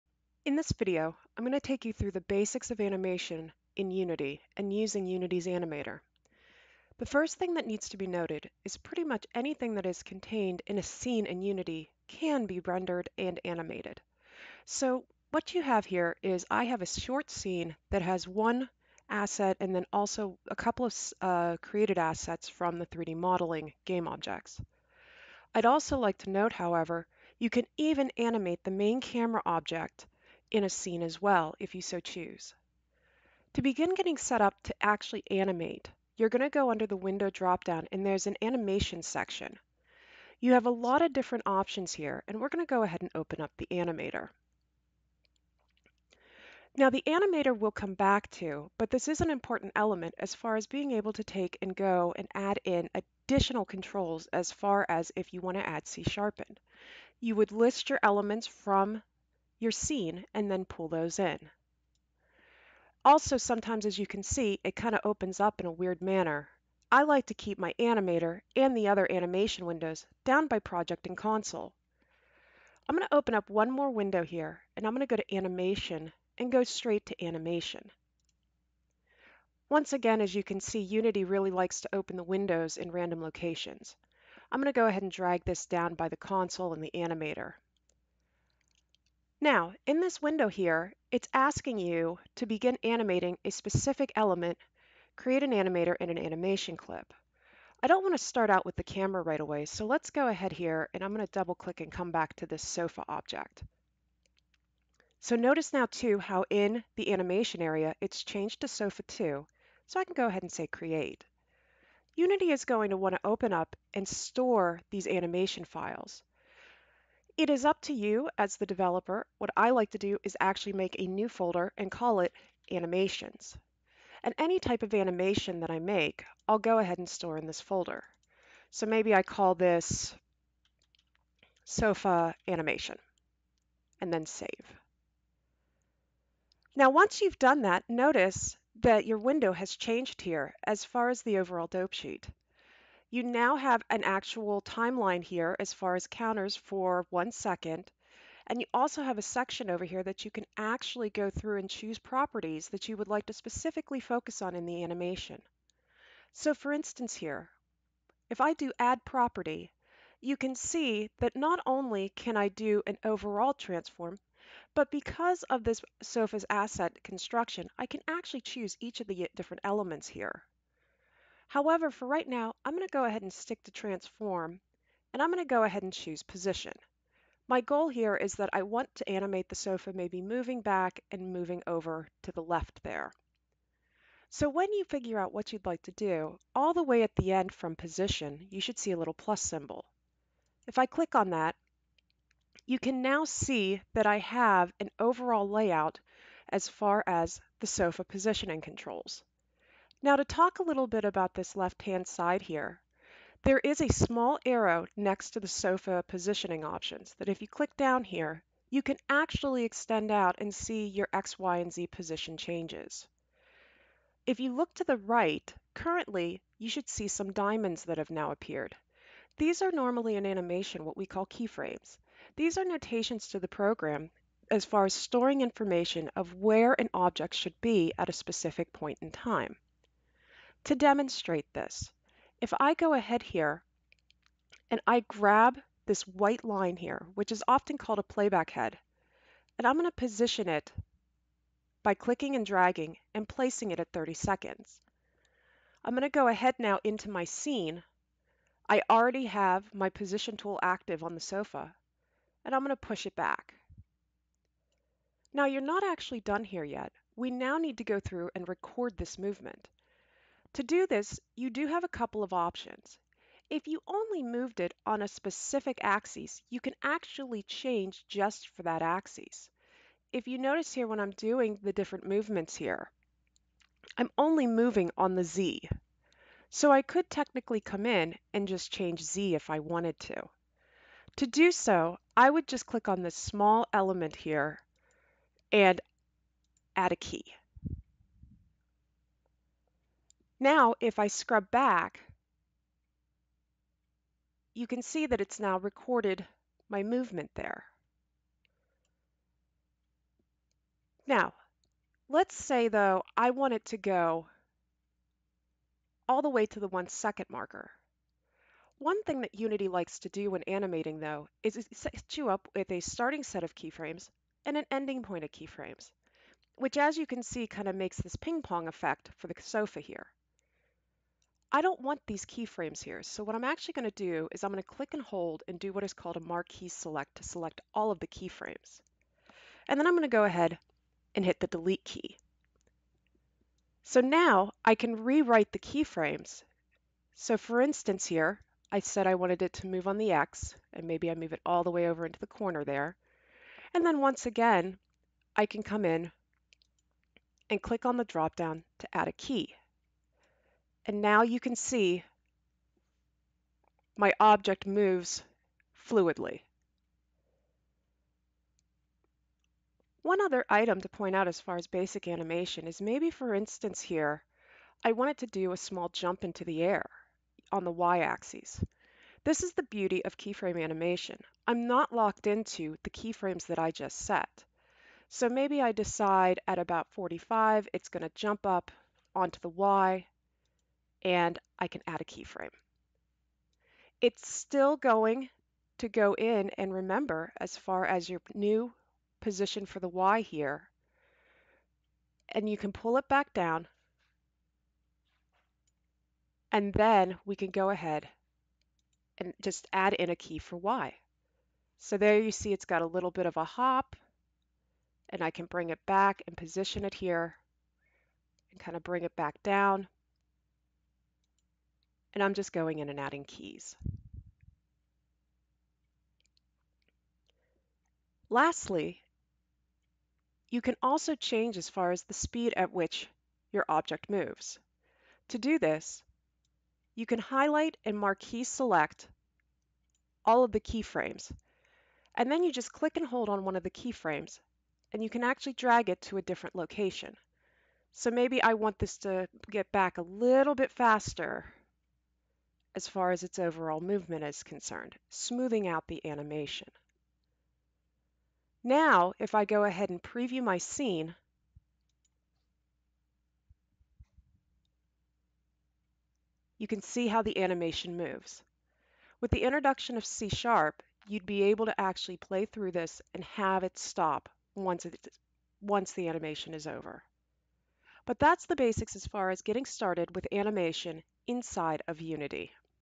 Basic Animation Option in Unity
Introduction to the Animation Panel in Unity working with an asset in a scene.